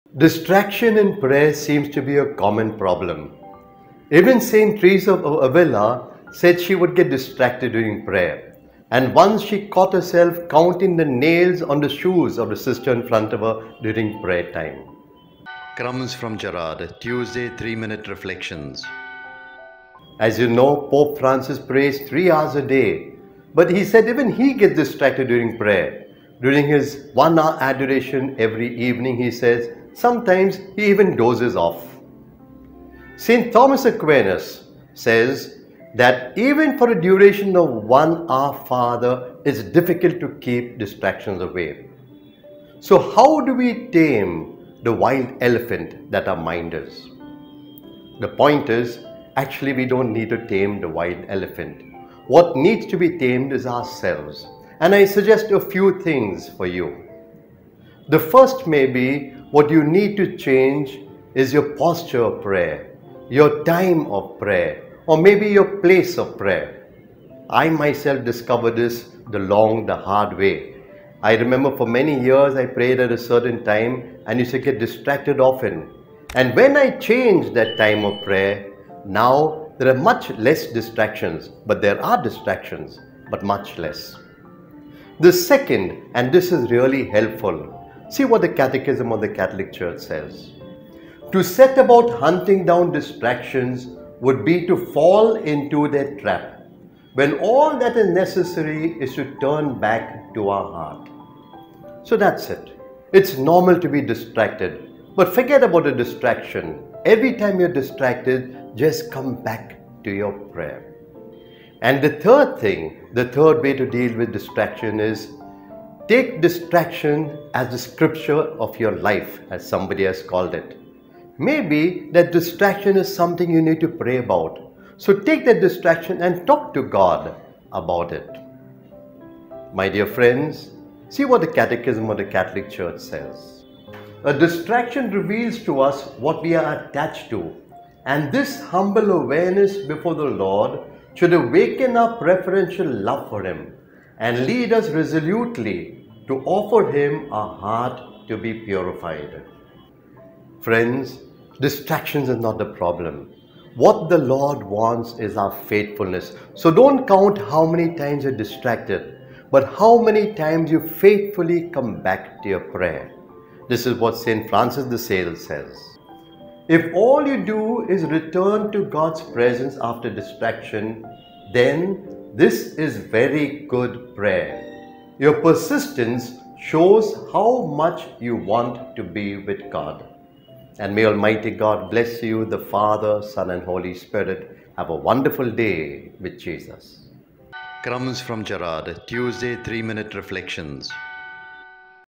How to deal with distraction in Prayer? | '3 Minute' Reflections
Our Mind is like a wild elephant...
| How to deal with distraction in Prayer?

SCRIPT
Distraction in prayer seems to be a common problem. Even St. Teresa of Avila said she would get distracted during prayer. Once, she caught herself counting the nails on the shoes of the sister in front of her during prayer time.
As you know, Pope Francis prays three hours a day, but he said even he gets distracted during prayer. During his one-hour adoration every evening, he says sometimes he even dozes off. St. Thomas Aquinas says that even for the duration of one Our Father, it is difficult to keep distractions away.
So, how do we tame the wild elephant that is our mind? The point is, actually, we don't need to tame the wild elephant. What needs to be tamed is ourselves, and I suggest a few things for you.
The first may be what you need to change is your posture of prayer, your time of prayer, or maybe your place of prayer. I myself discovered this the long, hard way. I remember for many years I prayed at a certain time and used to get distracted often. When I changed that time of prayer, now there are much fewer distractions. But there are distractions, but much less.
The second, and this is really helpful, see what the Catechism of the Catholic Church says: to set about hunting down distractions would be to fall into their trap when all that is necessary is to turn back to our heart. So, that's it. It's normal to be distracted, but forget about the distraction. Every time you're distracted, just come back to your prayer.
The third thing, the third way to deal with distraction, is to take distraction as the scripture of your life, as somebody has called it. Maybe that distraction is something you need to pray about. So, take that distraction and talk to God about it. My dear friends, see what the Catechism of the Catholic Church says: a distraction reveals to us what we are attached to, and this humble awareness before the Lord should awaken our preferential love for Him and lead us resolutely to offer Him a heart to be purified.
Friends, distraction is not the problem. What the Lord wants is our faithfulness. So, don't count how many times you're distracted, but how many times you faithfully come back to your prayer. This is what St. Francis de Sales says: if all you do is return to God's presence after distraction, then this is very good prayer. Your persistence shows how much you want to be with God.
And may Almighty God bless you, the Father, Son, and Holy Spirit. Have a wonderful day with Jesus.

Hi! I'm Fr Gerard Rodricks sj (Bombay Jesuit)
gerard sj